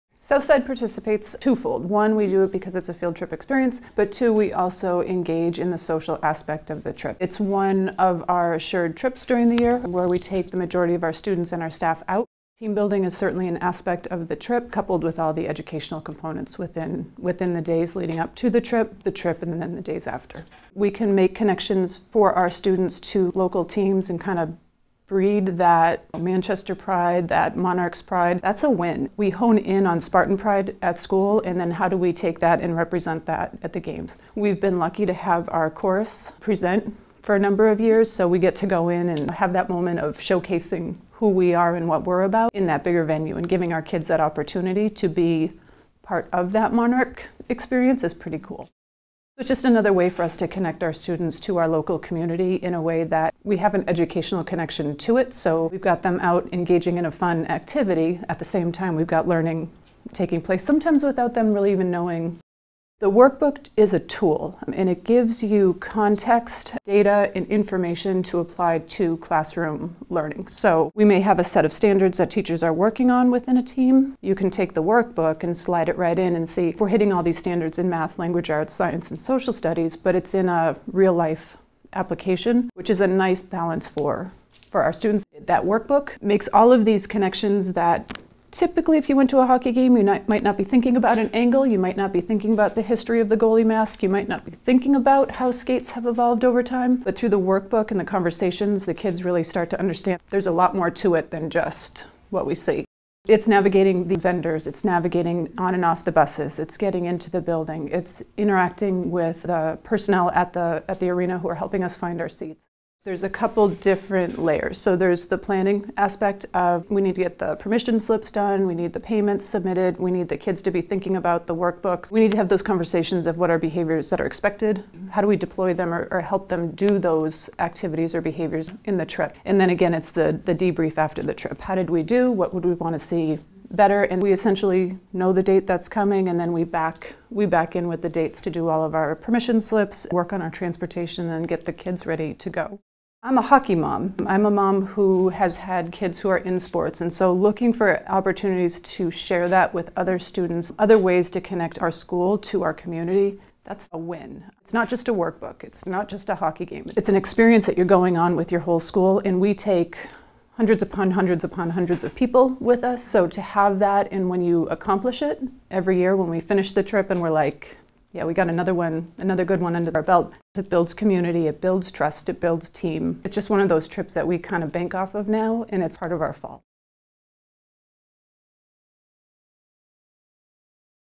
Monarchs Education Day 2017
Thank you to Jen Gillis, Principal of Southside Middle School in Manchester, NH for sharing her thoughts on our annual Education Day game!